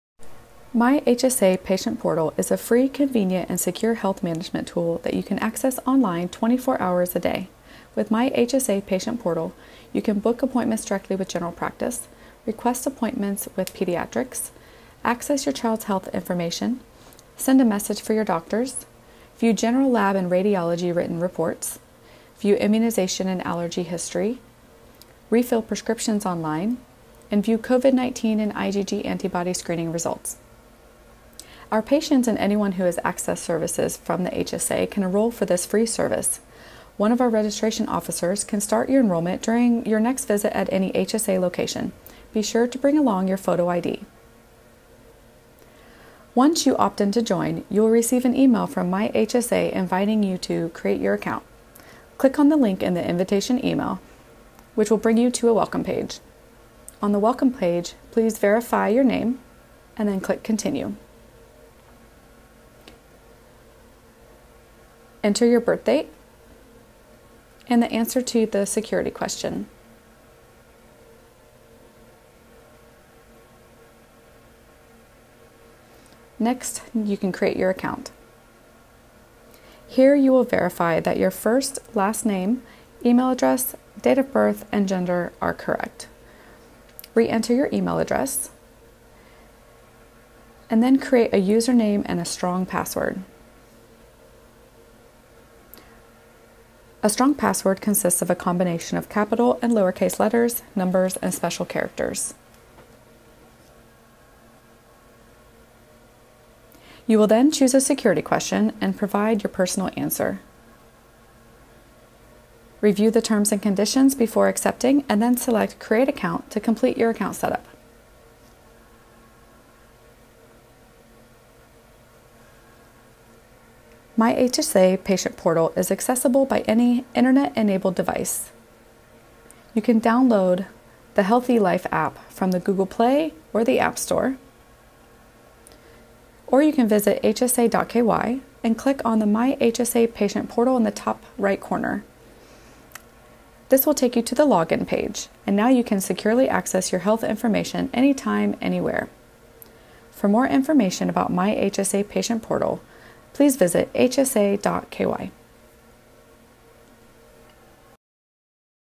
How to set up a MyHSA Patient Portal account
MyHSA is the Cayman Islands Health Services Authority’s online patient portal that gives you secure access to your health information 24-hours a day, 7 days a week. Sign-up for MyHSA Patient Portal takes less than a minute at any of our registration desks throughout the HSA and our District Health Centres. This video will take patients through the steps of setting up your account.